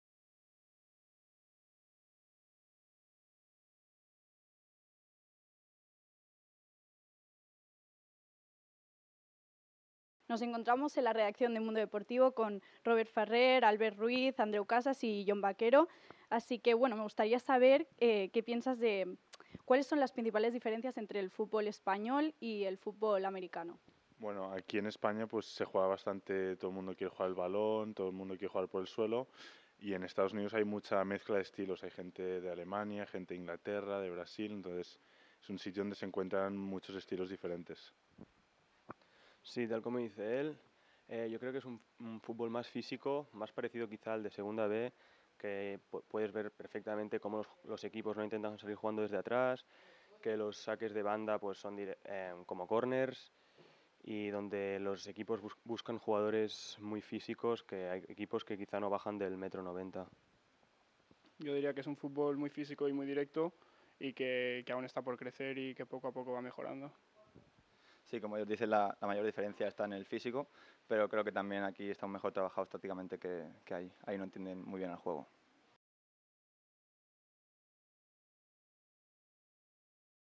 Españoles en la NCAA Soccer USA
Albert Ruiz, Andreu Cases, Robert Ferrer y Jon Bakero, jugadores de Soccer USA, explican las diferencias entre el fútbol español y el fútbol americano.